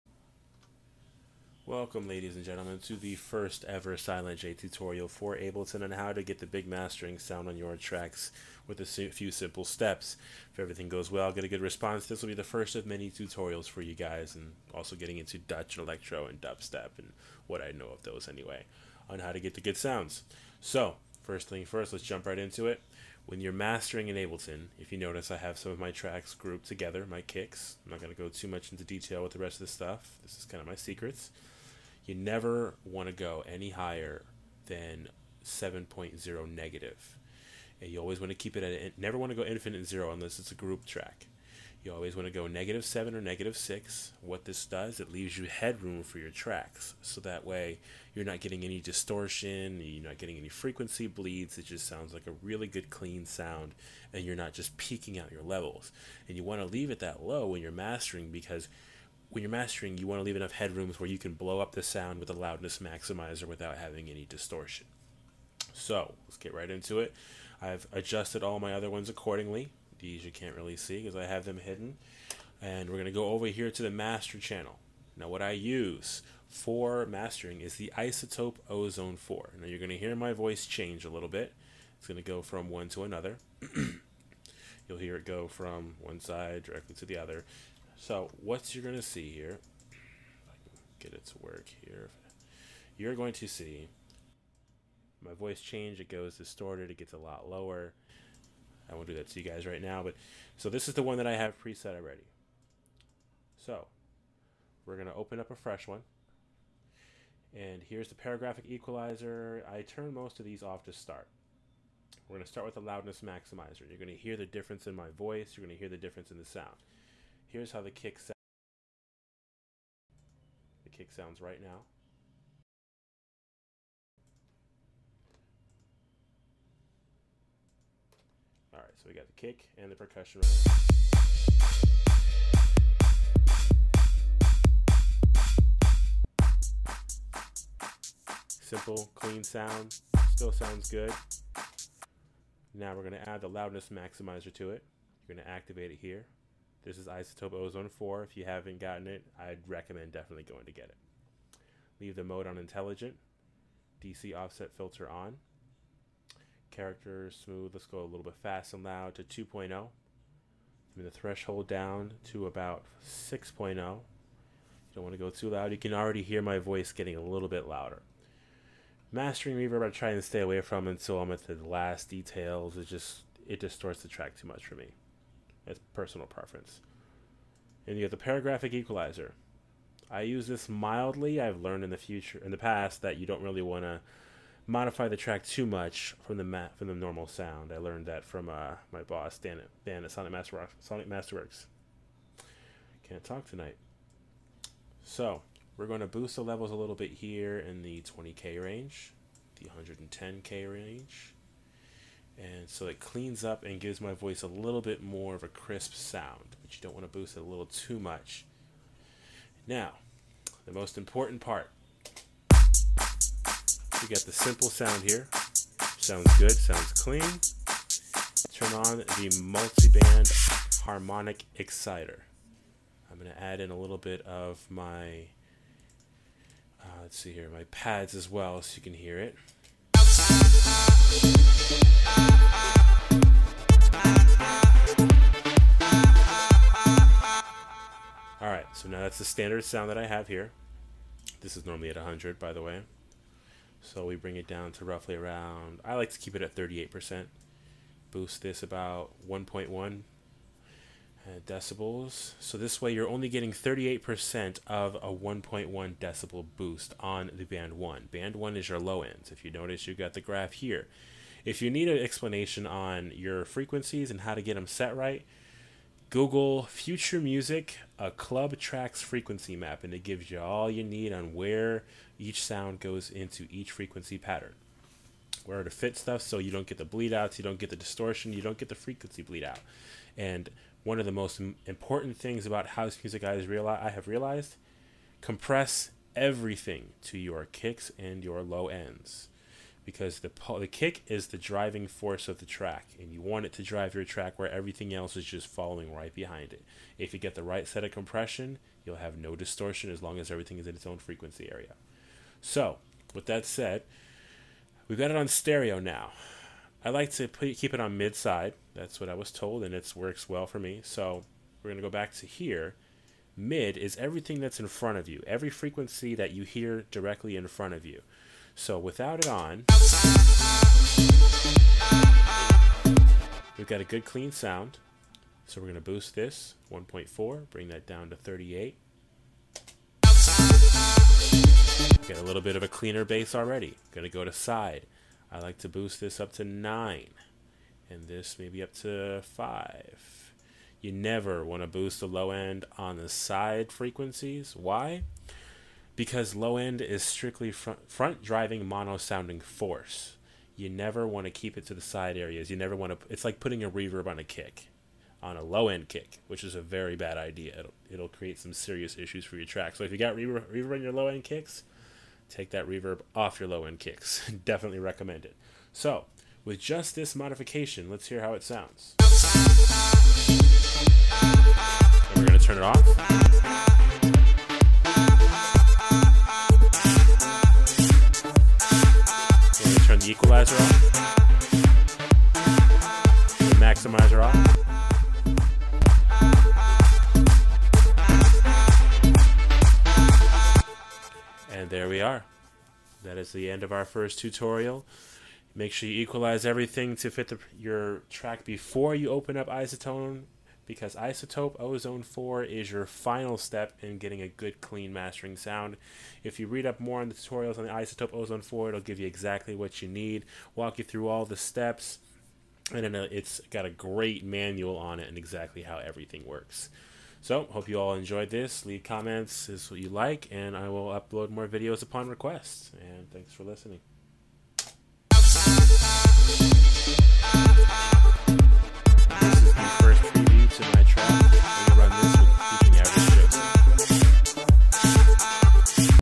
How to get a Clean Mastering Sound in Ableton
Using ableton and Izotope Ozone 4, i discuss how to get a good mastering sound on your track without distortion!